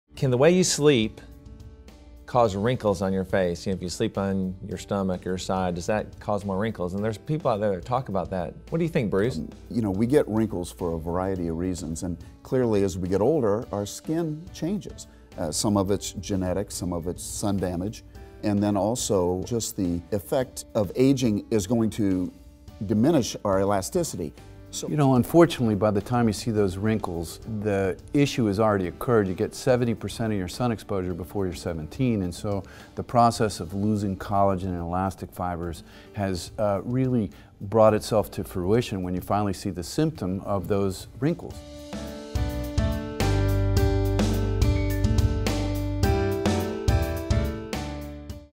Roundtables Sleeping and Wrinkles Sizzle Reel
Dr. Bruce Van Natta of Meridian Plastic Surgeons weighs in on the topic of whether your sleep position causes facial aging.